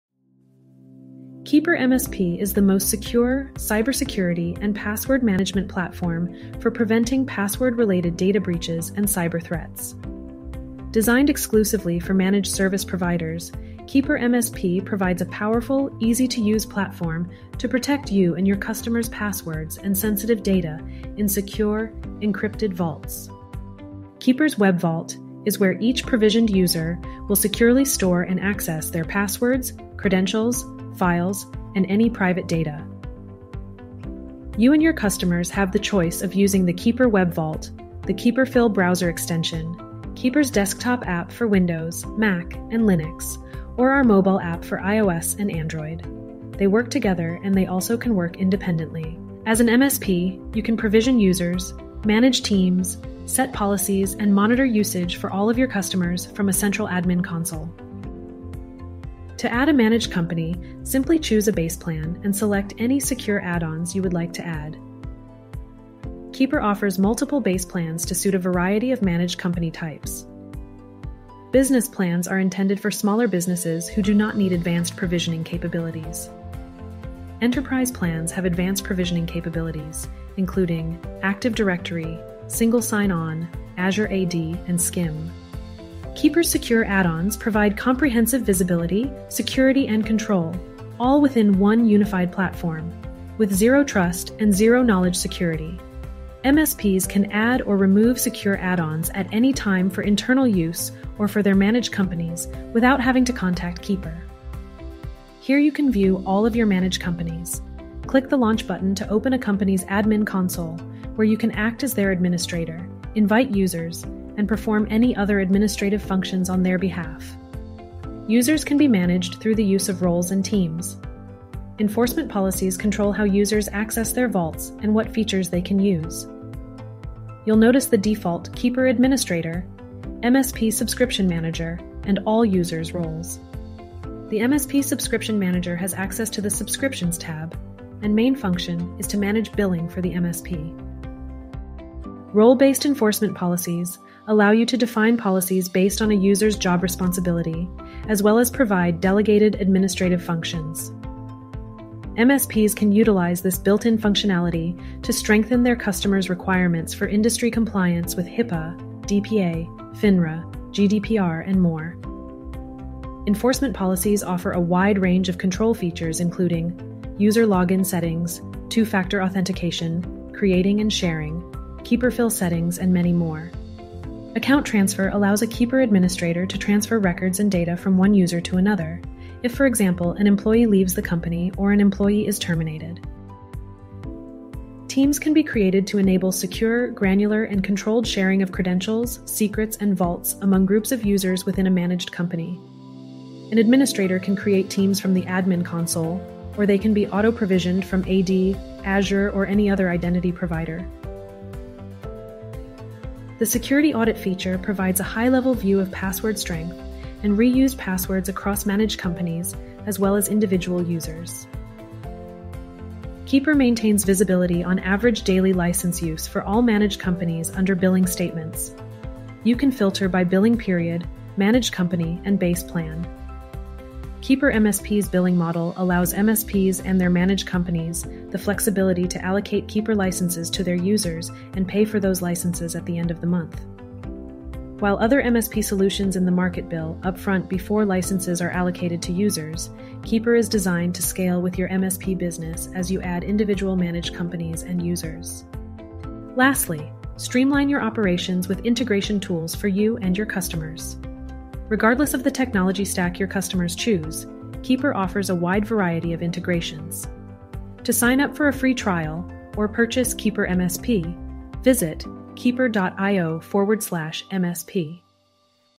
KeeperMSP
KeeperMSP is the most secure, cybersecurity and password management platform for preventing password-related data breaches and cyberthreats. Designed exclusively for Managed Service Providers, KeeperMSP provides a powerful, easy-to-use platform to protect you and your customers’ passwords and sensitive data in secure, encrypted vaults.

Web Vault
Keeper’s Web Vault is where each provisioned user will securely store and access their passwords, credentials, files and any private data.

You and your customers have the choice of using the Keeper Web Vault, the KeeperFill Browser Extension, Keeper’s Desktop App for Windows, Mac, and Linux, or our mobile app for iOS and Android. They work together and they also can work independently.

Admin Console
As an MSP, you can provision users, manage teams, set policies and monitor usage for all of your customers from a central Admin Console.

Base Plans & Secure Add-Ons
To add a Managed Company, simply choose a Base Plan and select any Secure Add-Ons you would like to add.

Keeper offers multiple base plans to suit a variety of managed company types. "Business" plans are intended for smaller businesses who do not need advanced provisioning capabilities. "Enterprise" plans have advanced provisioning capabilities, including: Active Directory, Single Sign On (SSO), Azure AD and SCIM.

Keeper's Secure Add-Ons provide comprehensive visibility, security and control, all within one unified platform - with zero-trust and zero-knowledge security. MSPs can add or remove Secure Add-Ons at any time for internal use or for their Managed Companies without having to contact Keeper.

Here you can view all of your Managed Companies. Click the launch button to open a company's Admin Console where you can act as their administrator, invite users, and perform any other administrative functions on their behalf.

Roles
Users can be managed through the use of Roles and Teams. Enforcement policies control how users access their vault and what features that they can use.

You'll notice the default "Keeper Administrator" and "MSP Subscription Manager" roles. The MSP Subscription Manager has access to the “Subscriptions” tab and main function is to manage billing for the MSP.

Enforcement Policies
Role-Based Enforcement Policies allow you to define policies based on a user's job responsibility as well as provide delegated administrative functions. MSPs can utilize this built-in functionality to strengthen their customers’ requirements for industry compliance with HIPAA, DPA, FINRA, GDPR and more.

Enforcement policies offer a wide-range of control features including: user login settings, two-factor authentication, sharing and uploading, KeeperFill settings and many more.

Account Transfer
Account Transfer allows a Keeper Administrator to transfer records and data from one user to another, if for example, an employee leaves the company or the employee is terminated.

Teams
Teams can be created to enable secure, granular and controlled sharing of credentials, secrets and vaults among groups of users within a managed company. An administrator can create teams from the Admin Console or they can be auto-provisioned from AD, Azure or any other identity provider.

Security Audit Overview
The Security Audit feature provides a high-level view of password strength and reused passwords across Managed Companies as well as individual users.

Billing & Invoicing
Keeper maintains visibility on average daily license use for all Managed Companies under Billing Statements. You can filter by billing period, managed company and base plan.

KeeperMSP’s billing model allows MSPs and their managed companies the flexibility to allocate Keeper licenses to their users and pay for those licenses at the end of the month. While other MSP solutions in the market bill upfront before licenses are allocated to users, Keeper is designed to scale with your MSP business as you add individual managed companies and users.

Integrations
Lastly, streamline your operations with integration tools for you and your customers. Regardless of the technology stack your customers choose, Keeper offers a wide variety of integrations.